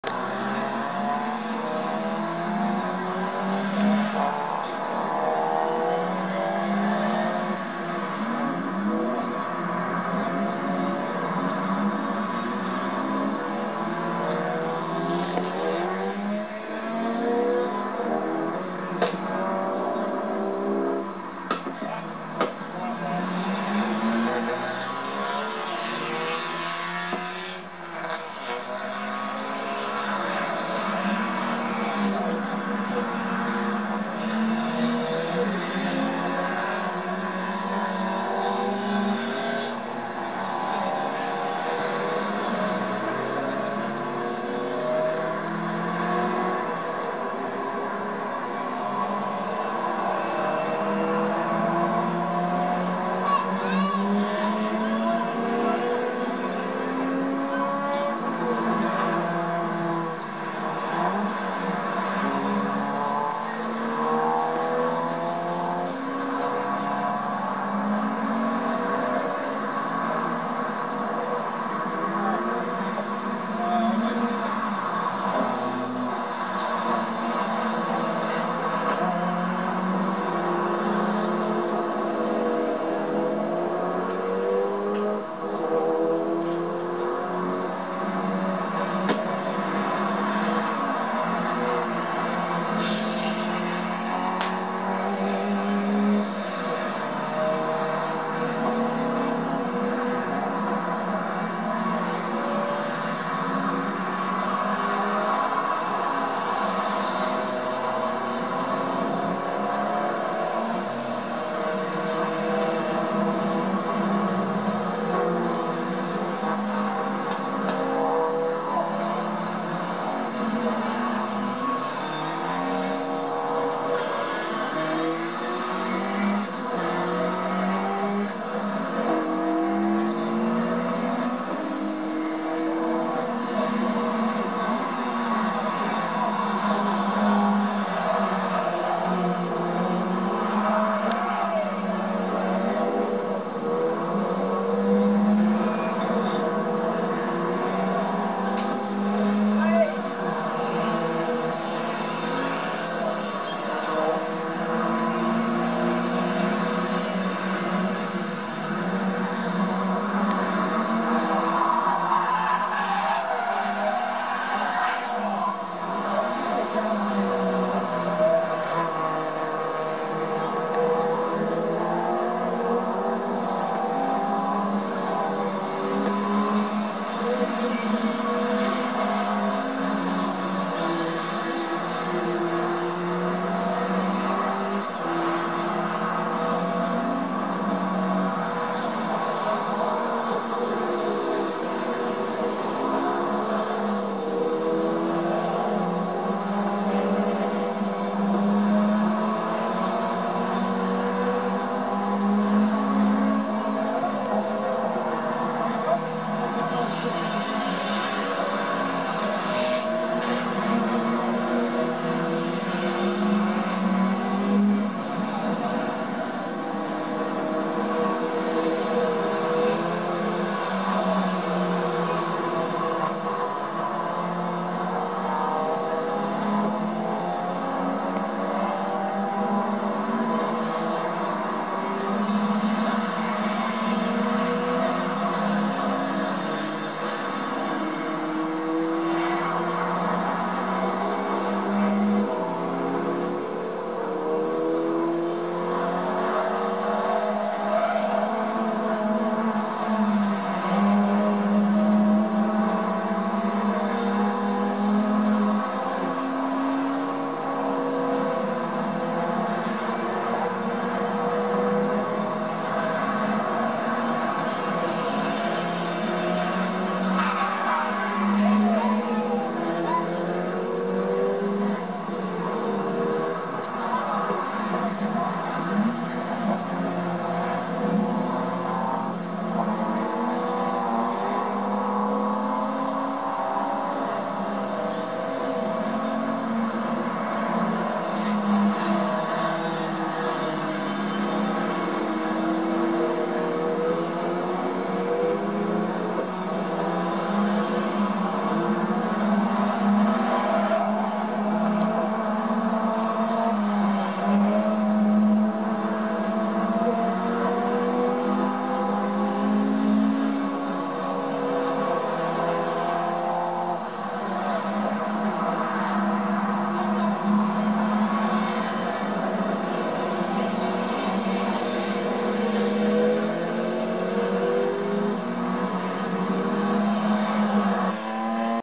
James Knight-Coney,Test session 4 Lydden Hill. British Rallycross Stock Hatch. 07/11/09
This video follows James Knight-Coney. It is showing Test session 4 at Lydden Hill. Driving his new British Rallycross Stock Hatch car. James Coney, the son of legendry Rally Cross driver Paul Coney, Starting out on his rally Cross adventure. In assosciation with WLS Racing.